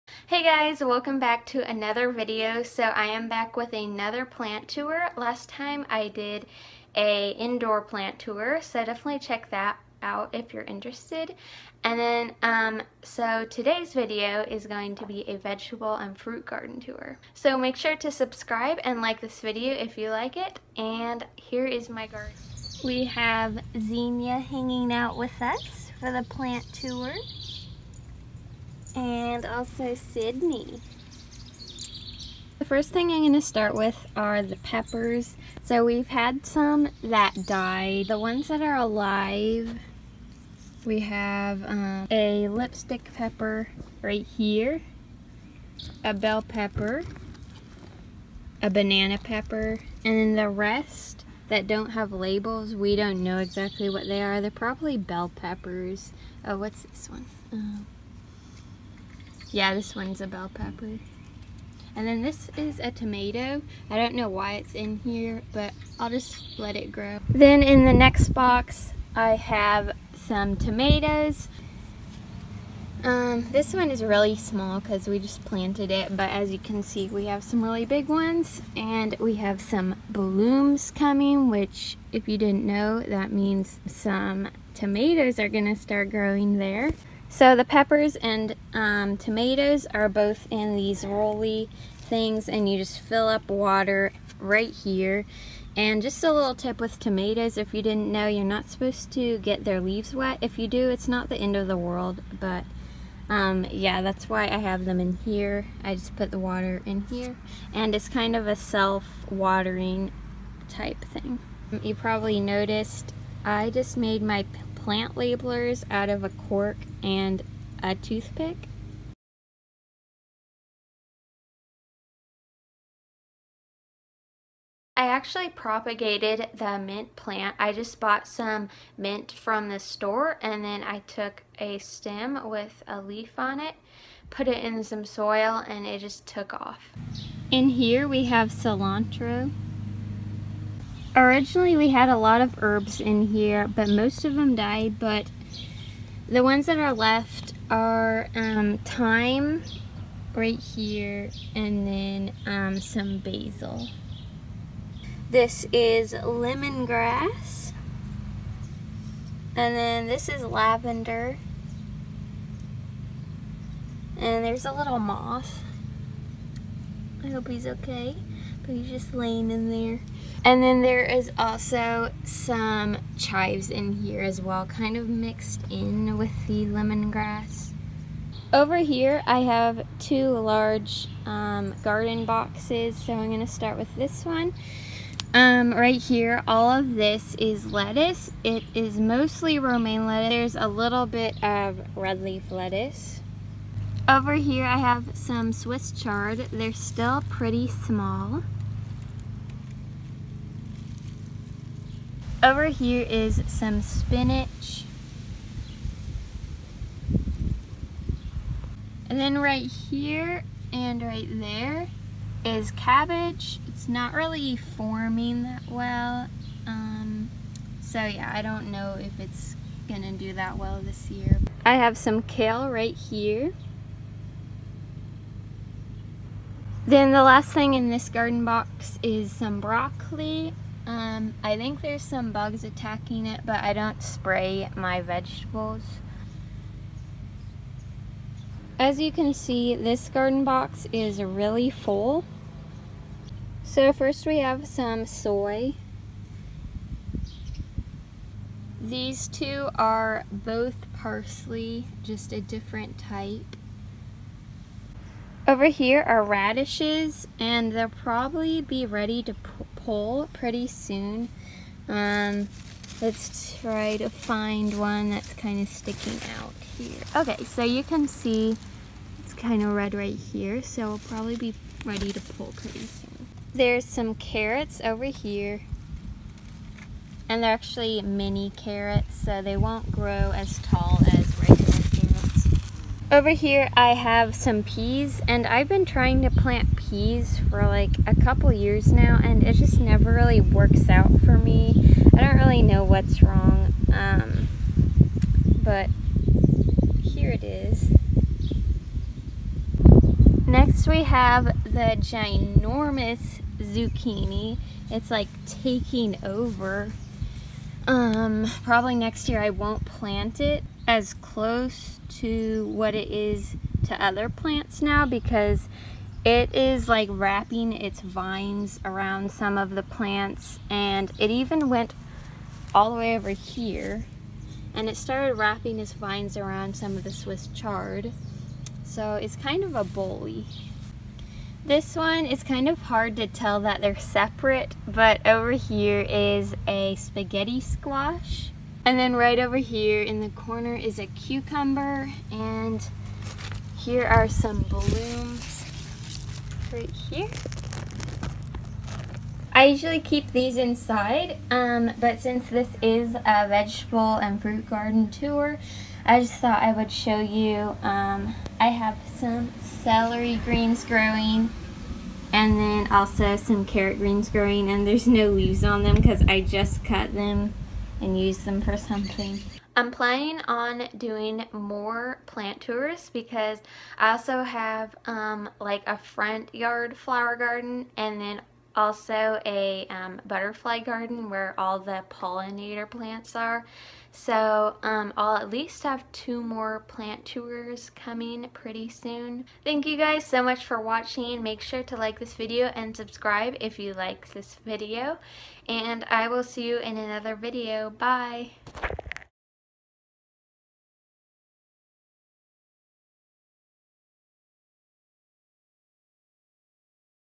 FRUIT AND VEGETABLE GARDEN TOUR // EDIBLE GARDEN (HERBS, VEGGIES, AND FRUIT)
Today I am giving you a fruit and vegetable garden tour. I have herbs, veggies and fruit in my garden. I made a indoor plant tour a few weeks ago and I plan to do more plant and garden videos.